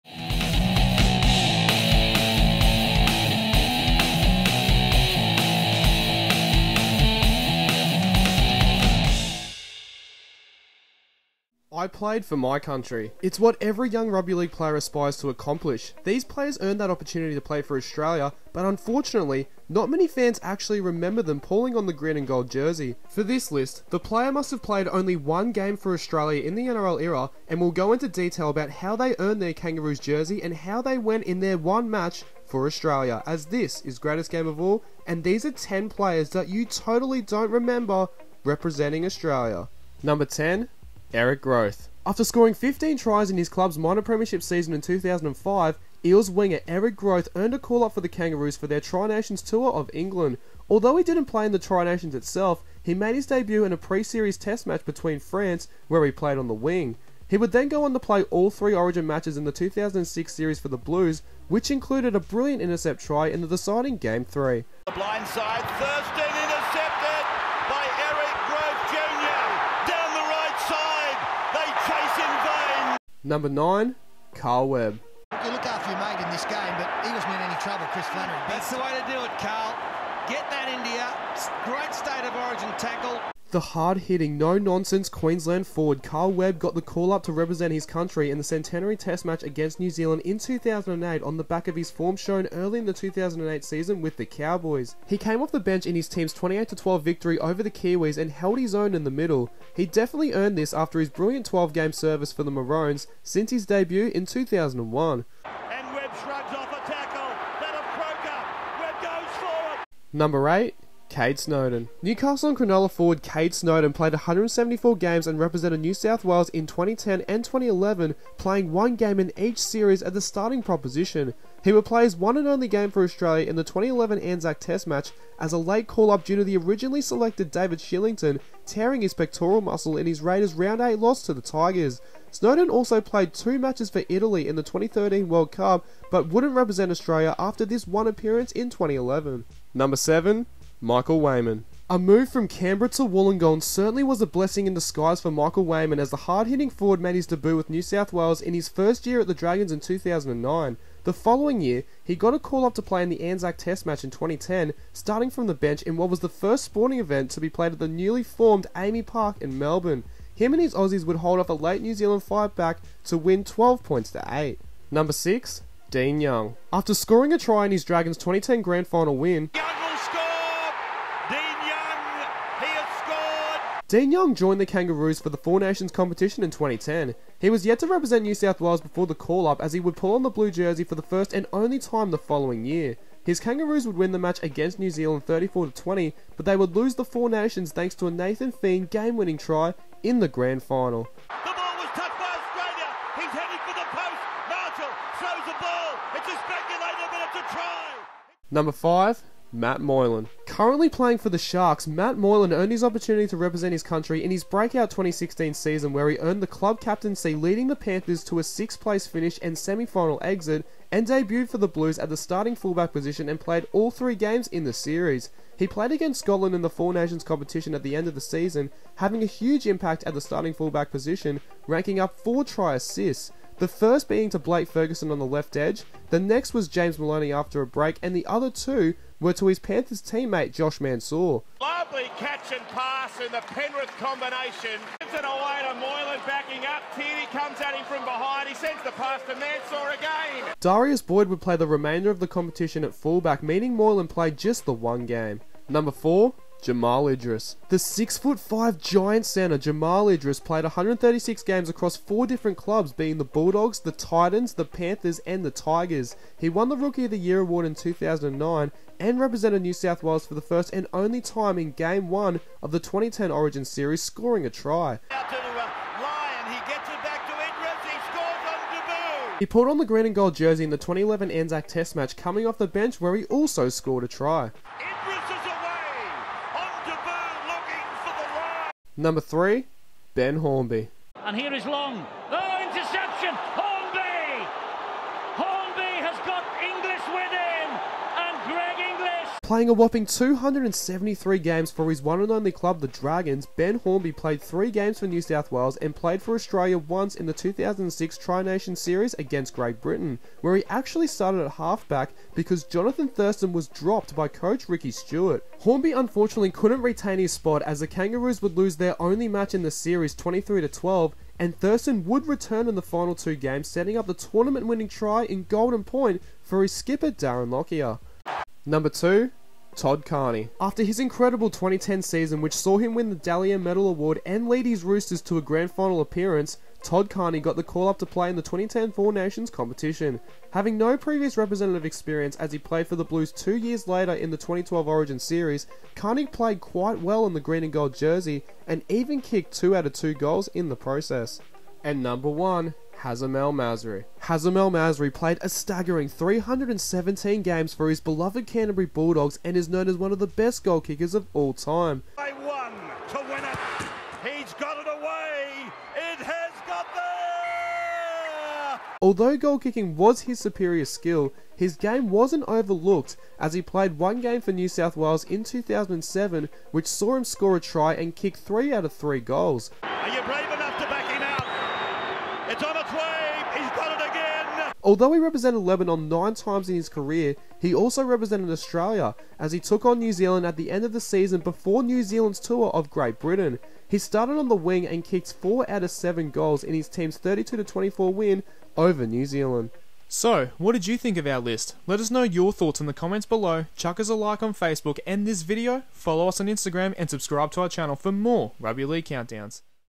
10 Players That You Don’t Remember Representing Australia (NRL)
I played for my country, it’s what every young rugby league player aspires to accomplish. These players earned that opportunity to play for Australia, but unfortunately, not many fans actually remember them pulling on a green and gold jersey. For this list, the player must have played only one game for Australia in the NRL era and we will go into detail about how they earned their Kangaroos jersey and how they went in their one match for Australia.

Full list of players who represented Australia in one game (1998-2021):

Jamie Ainscough
Terry Campese
Todd Carney (on list)
Joel Clinton
Jake Friend
Eric Grothe Jnr. (on list)
Aidan Guerra
Ben Hornby (on list)
Jamal Idris (on list)
Dallas Johnson
Alex Johnston
Antonio Kaufusi
Reni Maitua
Matt Moylan (on list)
Luke Priddis
Semi Radradra
Russell Richardson
Robbie Ross
Kade Snowden (on list)
Brett Stewart
Dave Taylor
Daniel Tupou
Carl Webb (on list)
Michael Weyman (on list)
Dean Young (on list)